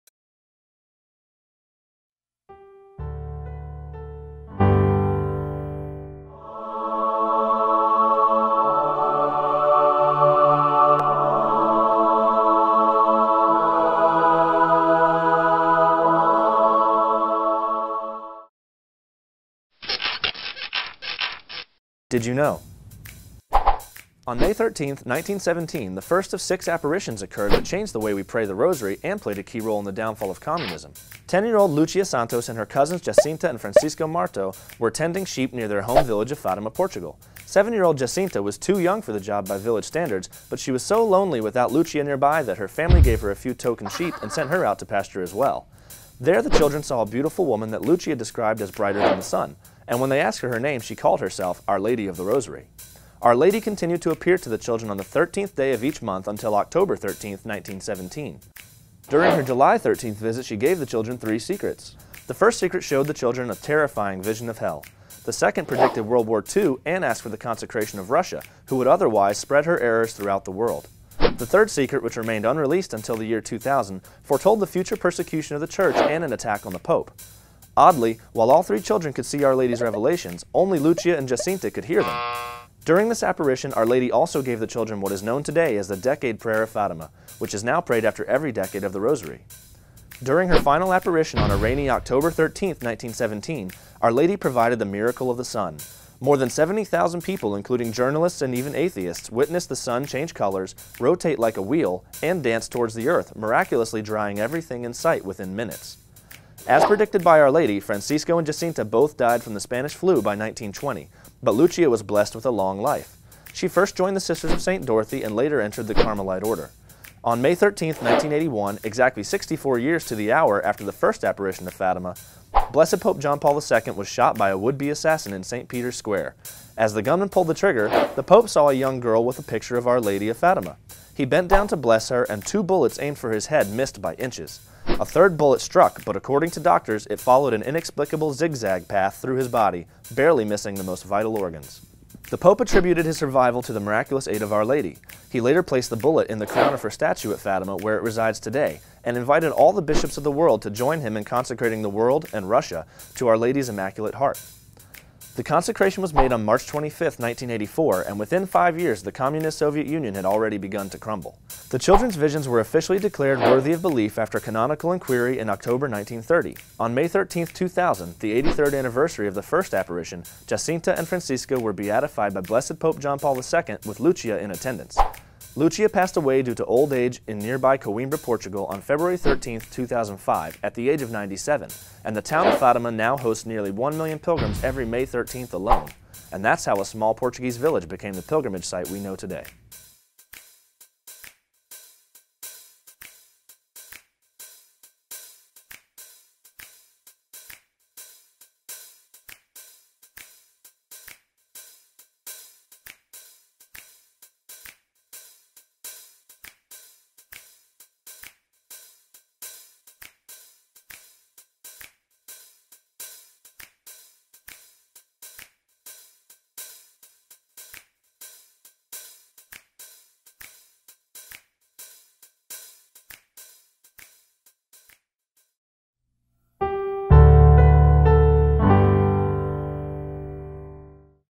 Did You Know? Fatima (Feast Day: May 13)
The fourth episode in an ongoing series of brief videos filled with interesting details regarding the lives and miracles of well-known and lesser-known saints, and the devotions and traditions that have developed around them over time.

More "Did You Know?":
 - St. Joseph (Feast Day: March 19)
 - St. Bernadette Soubirous (Feast Day: April 16)
 - St. Catherine of Siena (Feast Day: April 29)

TRANSCRIPT
Fátima

On May 13, 1917, the first of six apparitions occurred that changed the way we pray the rosary and played a key role in the downfall of Communism.

Ten-year-old Lúcia Santos and her cousins Jacinta and Francisco Marto were tending sheep near their home village of Fátima, Portugal. Seven-year-old Jacinta was too young for the job by village standards, but she was so lonely without Lúcia nearby that her family gave her a few token sheep and sent her out to pasture, too. There, the children saw a beautiful woman that Lúcia described as "brighter than the sun" and when they asked her name, she called herself "Our Lady of the Rosary".

Our Lady continued to appear to the children on the 13th day of each month until October 13th, 1917. During her July 17th visit she gave the children "Three Secrets". The First Secret showed the children a terrifying vision of Hell. The Second predicted World War II, and asked for the Consecration of Russia, who would otherwise "spread her errors throughout the world." The Third Secret, which remained unreleased until the year 2000, foretold the future persecution of the Church and an attack on the Pope.

Oddly, while all three children could see Our Lady's revelations, only Lúcia and Jacinta could hear them. During this apparition Our Lady also gave the children what is known today as the Decade Prayer of Fátima, which is now prayed after every decade of the rosary.

During her final apparition on a rainy October 13, 1917, Our Lady provided the "Miracle of the Sun". More than 70,000 people, including journalists and even atheists, witnessed the sun change colors, rotate like a wheel and dance towards the Earth, miraculously drying everything in sight within minutes.

As predicted by Our Lady, Francisco and Jacinta both died from the Spanish Flu by 1920, but Lúcia was blessed with a long life. She first joined the Sisters of St. Dorothy, and later entered the Carmelite order.

On May 13, 1981, exactly 64 years to the hour after the first apparition of Fátima, Blessed Pope John Paul II was shot by a would-be assassin in St. Peter's Square. As the gunman pulled the trigger, the Pope saw a young girl with a picture of Our Lady of Fátima. He bent down to bless her and missed two bullets aimed for his head by inches. A third bullet struck. But according to doctors, it followed an inexplicable zig-zag path through his body, barely missing the most vital organs.

The Pope attributed his survival to the miraculous aid of Our Lady.  He later placed the bullet in the crown of her statue at Fátima, where it resides today, and invited all the bishops of the world to join him in consecrating the world -- and Russia -- to Our Lady's Immaculate Heart.

The consecration was made on March 25, 1984 and within five years the Communist Soviet Union had already begun to crumble.

The children's visions were officially declared "worthy of belief" after canonical enquiry in October 1930. On May 13th, 2000, the 83rd anniversary of the first apparition, Jacinta and Francisco were beatified by Blessed Pope John Paul II with Lúcia in attendance. Lúcia passed away due to old age in nearby Coimbra, Portugal on February 13, 2005, at the age of 97 and the town of Fátima now hosts nearly one million pilgrims every May 13th alone.

And that's how a small Portuguese village became the pilgrimage site we know today.